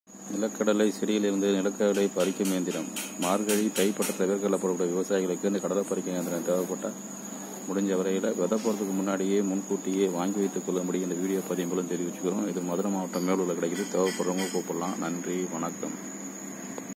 கடலைபரிக்கும் இயந்திரம் 9787672239
Groundnuts harvesting machines melur madurai d tamilnadu india six man work machine rs 39000